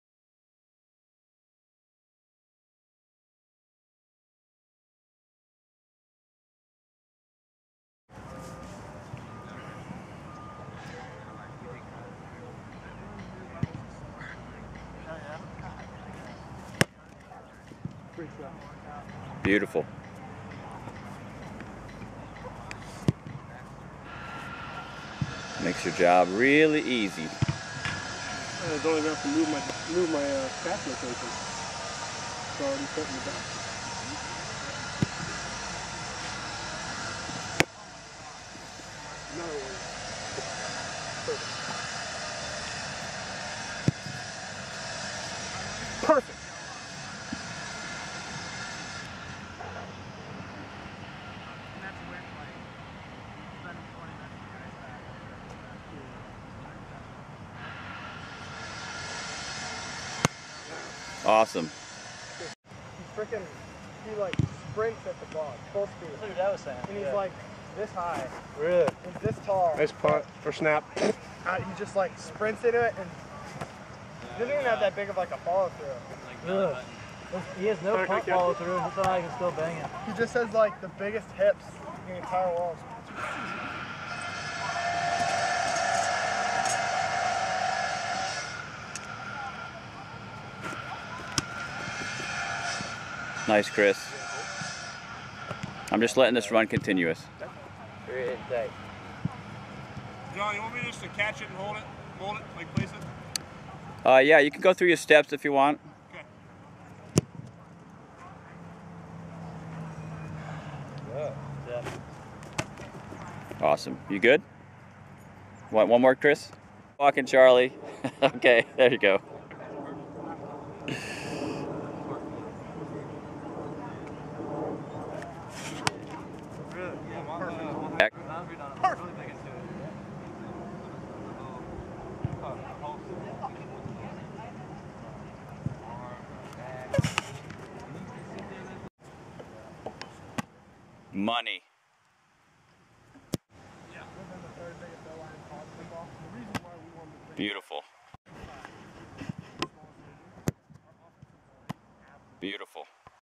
Chris Highland NFL Free Agent Long Snapper - 12/11/15
Chris Highland NFL Free Agent Long Snapper - 6'2'' 232 lbs

Chris spent a week in San Diego training with former NFL kicker John Carney. This video is from one of the workouts with John and other free agent kickers and punters. John says that Chris looks even better than he did in training camp with New Orleans.